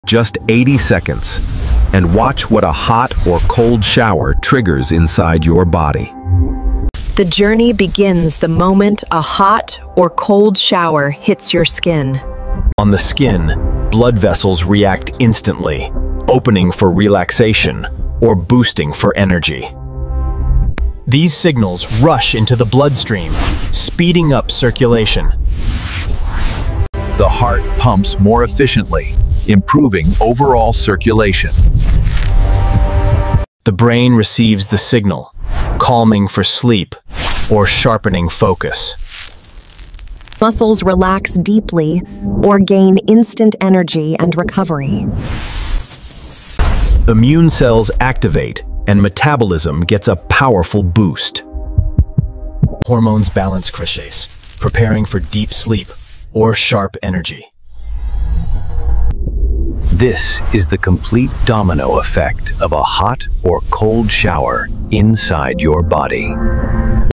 cold shawer vs hot shawer
Hot shower or cold shower? It's not about which is better, but WHEN to take each! 😱 In this stunning 3D animation, discover the shocking domino effect inside your body. A hot shower opens blood vessels, relaxes muscles, lowers stress, and prepares you for deep sleep. A cold shower boosts circulation, sharpens focus, fires up metabolism, and strengthens immunity. Watch the real-time chain reaction from skin to brain, heart, and beyond! Plus, learn about contrast showers that give you the benefits of BOTH. Backed by science – this could change your daily routine forever! 🚿💙